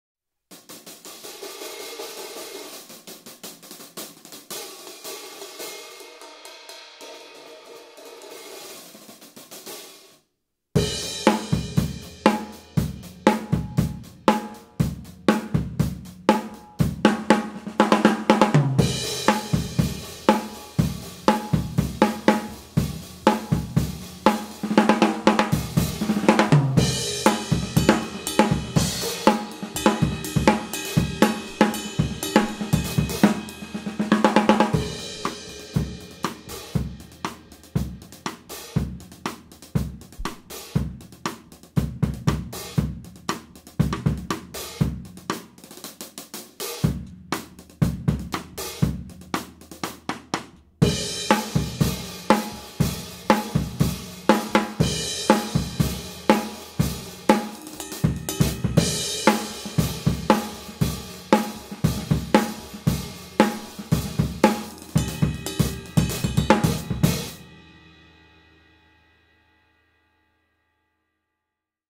Meinl 16" Byzance Extra Dry Medium Thin Hi Hat Cymbals - 962/1506g (B16EDMTH-1081417B)
*SOLD* Meinl 16" Byzance Extra Dry Medium Thin Hi Hat Cymbals available from myCymbal.com. See more cymbals like this

Other gear used in this video:
• Cymbals (From drummer's L to R): Meinl 18" Pure Alloy Traditional Medium Crash Cymbal, Meinl 20" Pure Alloy Traditional Medium Ride Cymbal, Meinl 16" Byzance Jazz Extra Thin Crash Cymbal